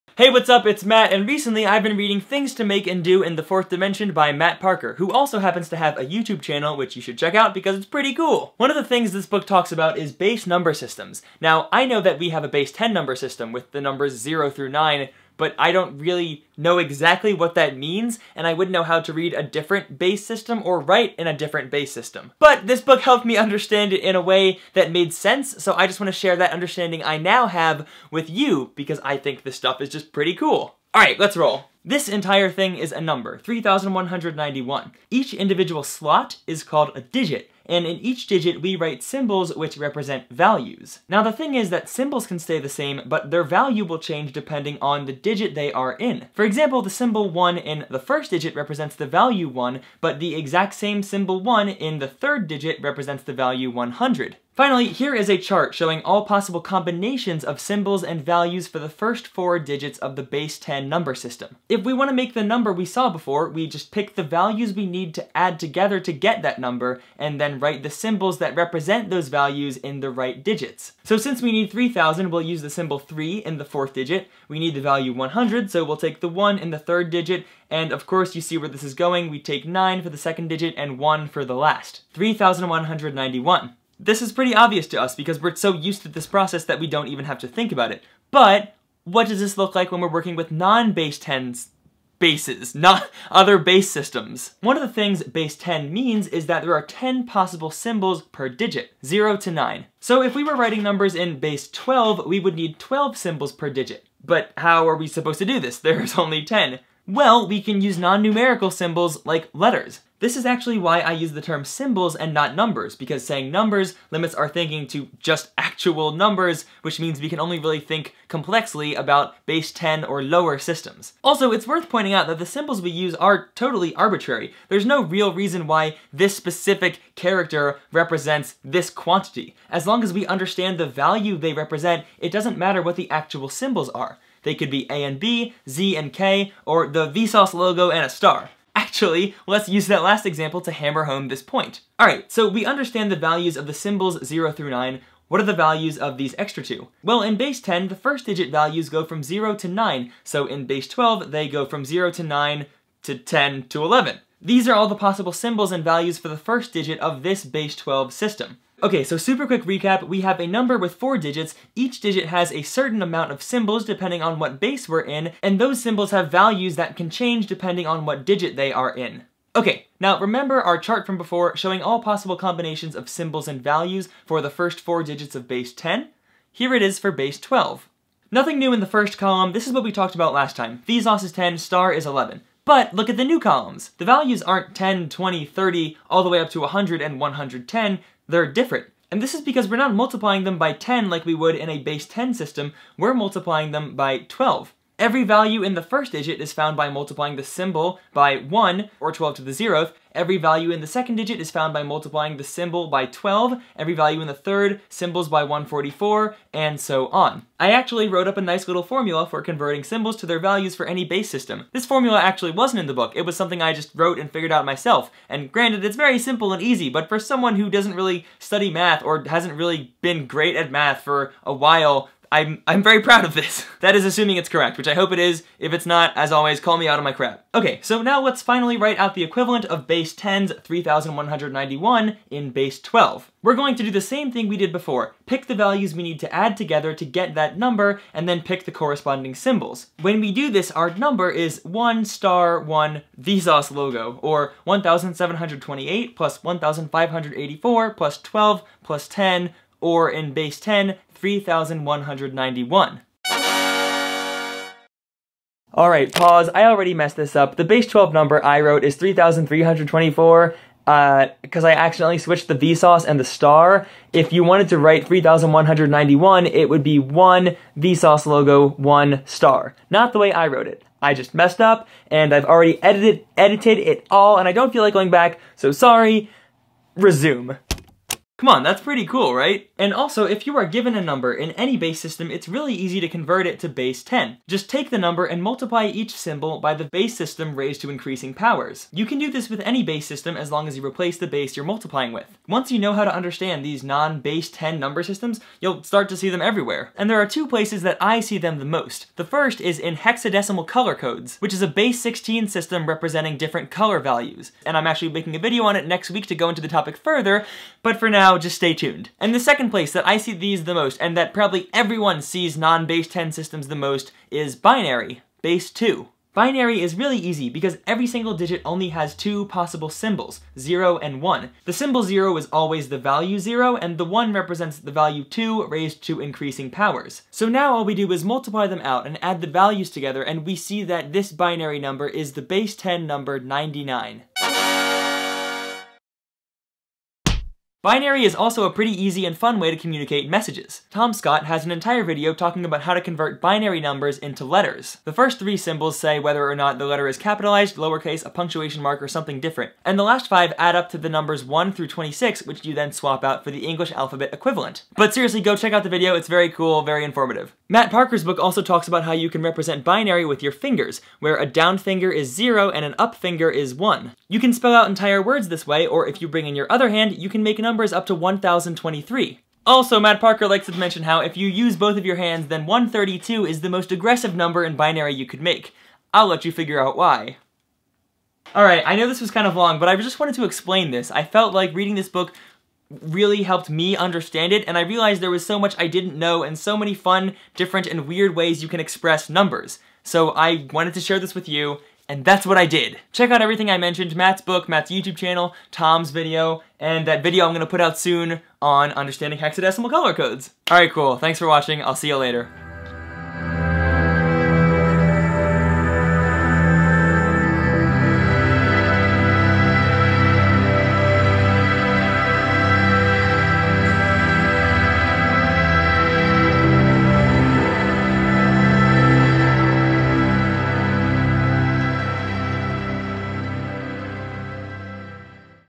Understanding Base Number Systems
What does base 10 mean? What do base number systems mean? I explain it here! (with some mistakes along the way)